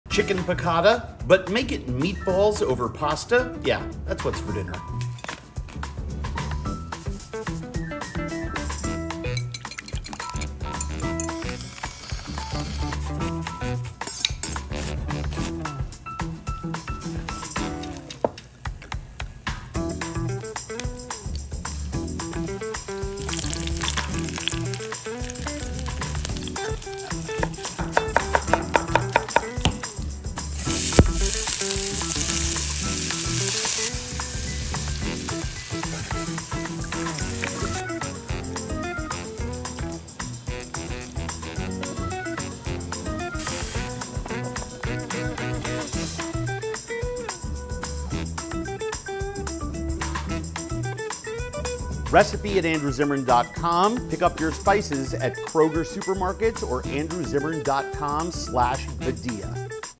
Chicken Piccata Meatballs with Badia
Steal this recipe for your next dinner party!

Lemony chicken piccata meatballs over angel hair or any pasta made with my Badia Spices Italian Seasoning, exploding with the rich flavors of basil, oregano, garlic, shallot and chiles.

Pick up my entire Badia Spice line at at your local @krogerco, or on bodega.badiaspices.com.

Make this dish and post your photos!